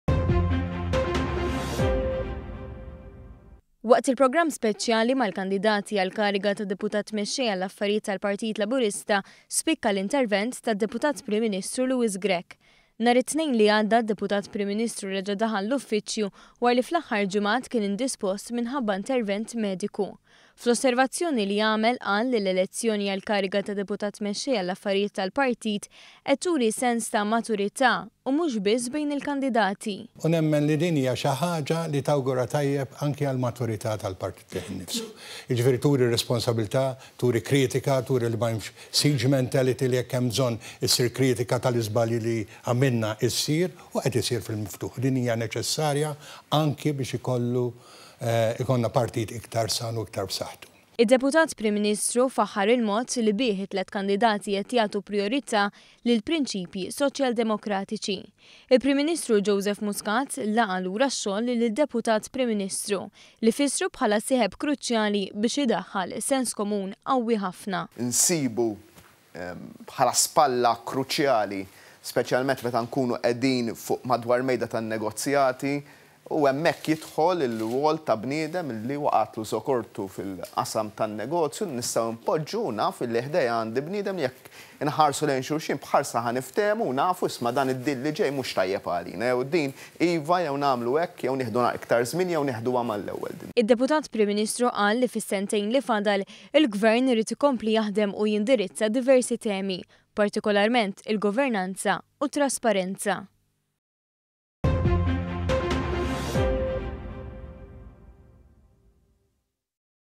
PARTIT LABURISTA B’SAĦĦTU.
ONE.COM.MT • L-elezzjoni għall-kariga tad-deputat mexxej qed turi sens ta’ maturita mhux biss bejn il-kandidati imma anki fi hdan l-partit laburista. Riflessjoni li ghamilha d-deputat Prim Ministru Louis Grech, li esprima l-ħeġġa li jkompli jahdem mat-team tal-partit laburista u mad-deputat mexxej li se jigi elett.